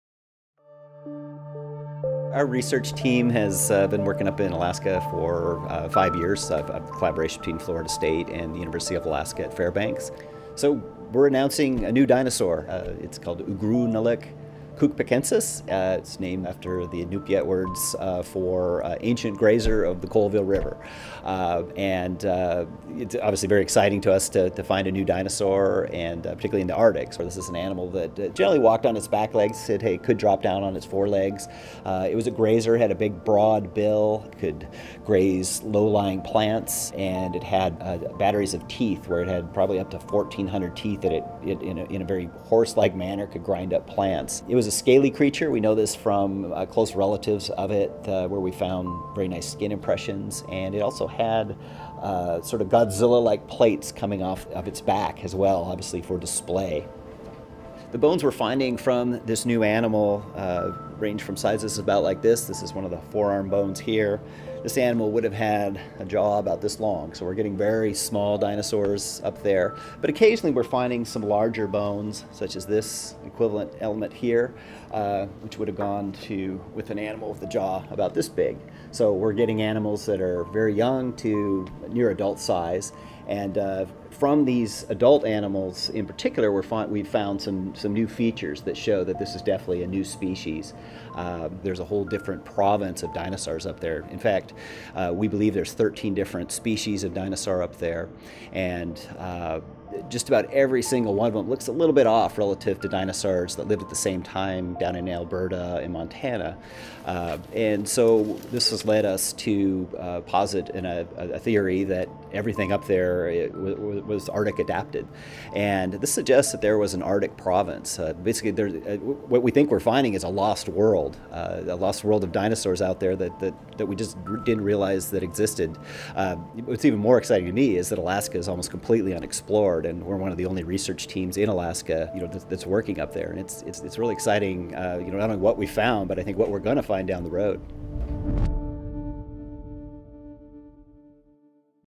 Research Team Discovers 'Lost World' of Cold Weather Dinosaurs
A collaborative team between Florida State University and the University of Alaska Fairbanks has spent the last five years digging in a remote bone-bed of dinosaur remains in the remote Prince Creek Formation in Alaska. FSU Professor Gregory Erickson is excited for the new species of duck-billed dinosaurs uncovered in the dig and believes it opens up a lost province of arctic adapted dinosaurs. The new dino, Ugrunaaluk kuukpikensis, is closely related to Edmontosauras, another duck-billed dinosaur found further south near Alberta, Montana, and South Dakota, but several structural differences in  adult skeletons helped Erickson and Pat Druckenmiller of the University of Alaska Fairbanks determine it is in fact a different species.